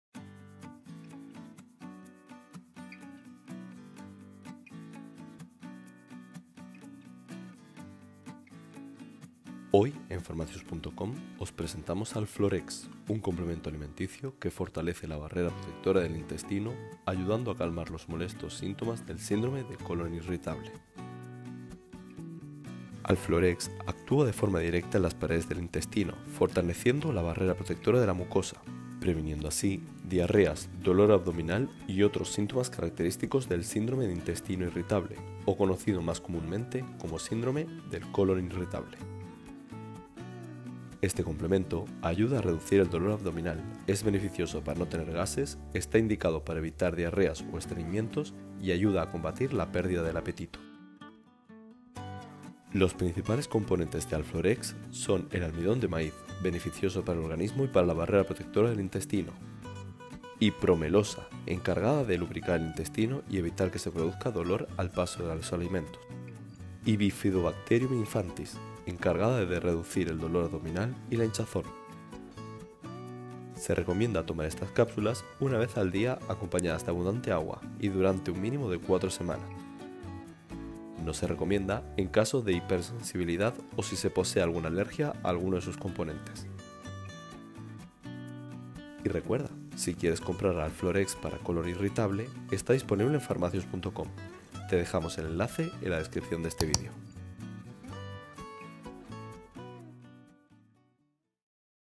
Alflorex: probiótico para el colon irritable
ALFLOREX 30 CÁPSULAS ►

Alflorex es un probiótico pensado para fortalecer la barrera protectora del intestino, ayudando así a calmar los síntomas del colón irritable.

00:00 Intro
00:09 Presentación: Alflorex Cápsulas
00:13 Qué es
00:23 Cómo actúa Alflorex
00:40 Para qué sirve
00:52 Composición
01:12 Cómo tomar Alflorex
01:29 Comprar Alforex en Pharmacius.com

¡Comparte tus opiniones sobre las cápsulas del probiótico Alflorex!